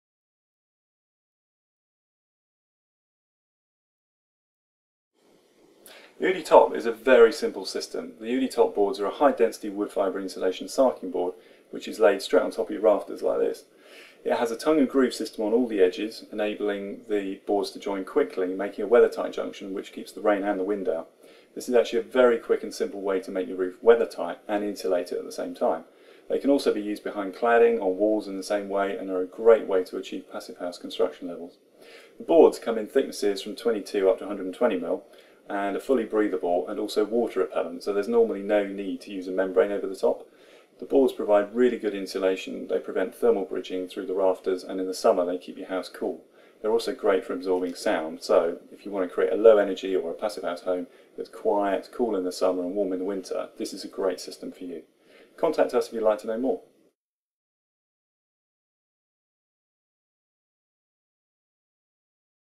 What is UdiTOP?
UdiTOP wood fibre roof insulation sarking board is a very simple system. The UdiTOP boards are a high density wood fibre insulation sarking board which is laid straight on top of your rafters. It has a tongue and groove system on all edges enabling the boards to join quickly, making a weathertight junction which keeps the rain and the wind out. This is actually a very quick and simple way to make your roof weathertight and insulate it at the same time. They can also be used behind cladding on walls in the same way and are a great way to achieve passivhaus construction levels.

The boards come in thicknesses from 22 to 120mm and are fully breathable and also water repellent so there is normally no need to use a membrane over the top. The boards provide really good insulation, they prevent thermal bridging through the rafters and in the summer they keep your house cool. They’re also great for absorbing sound so if you want to create a low energy or passivhaus home that is quiet, cool in the summer and warm in winter this is a great system for you.